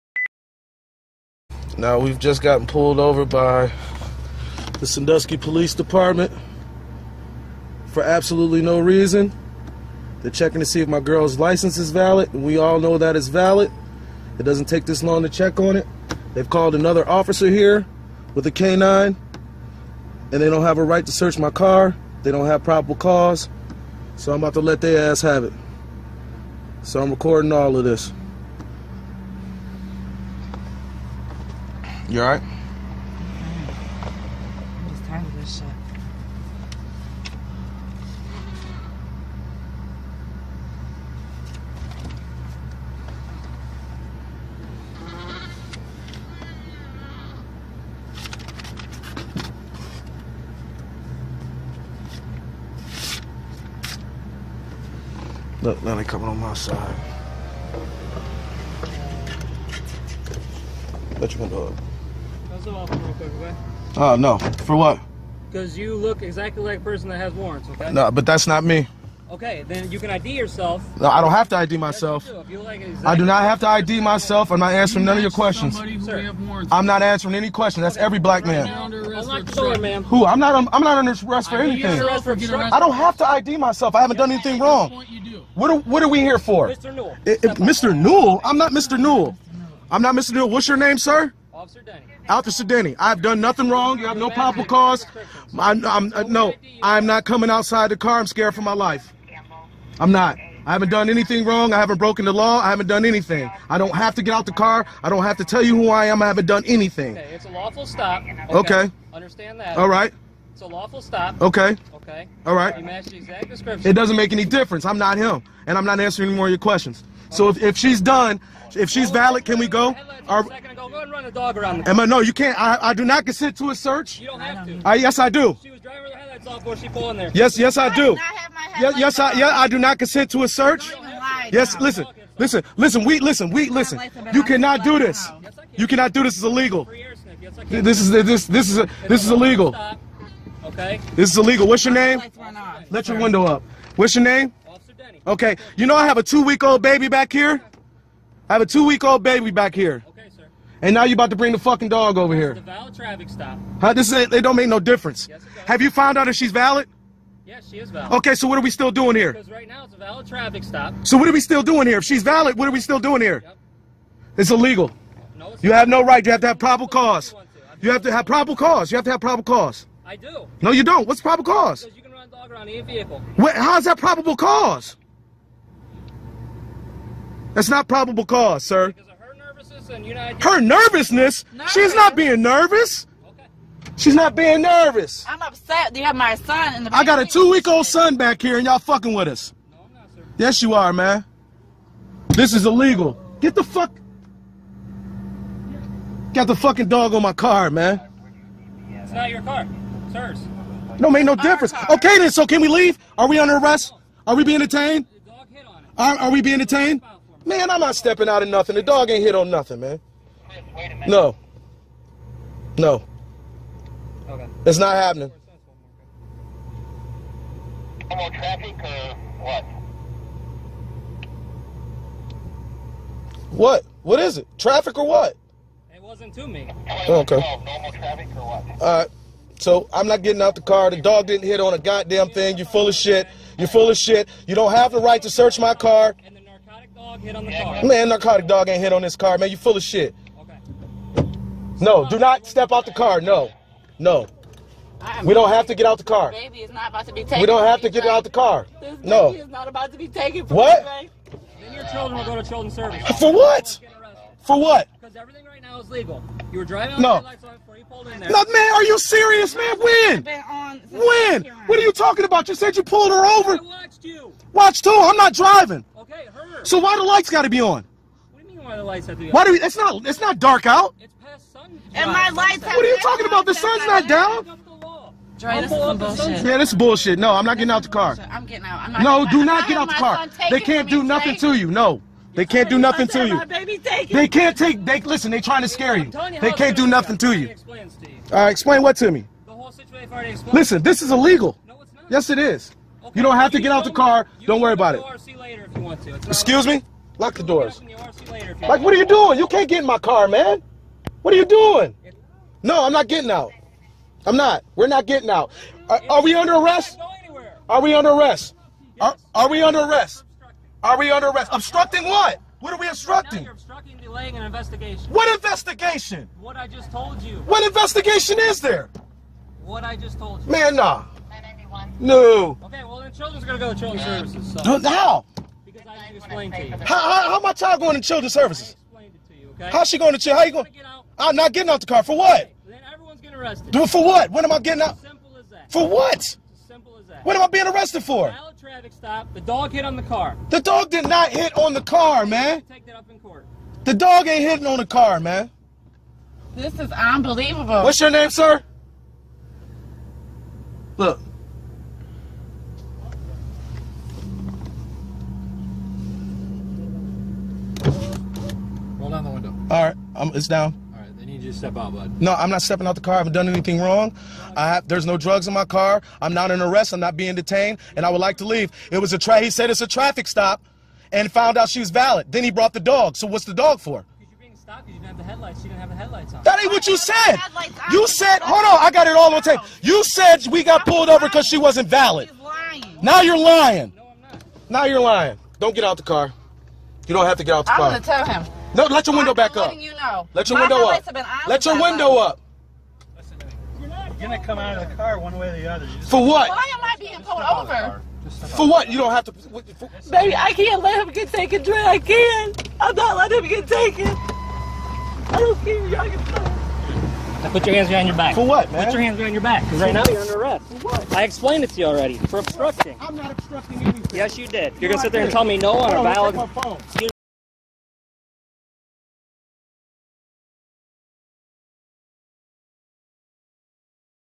Andre Stockett and Sandusky Police
Traffic stop involving the Sandusky police department and Andre Stockett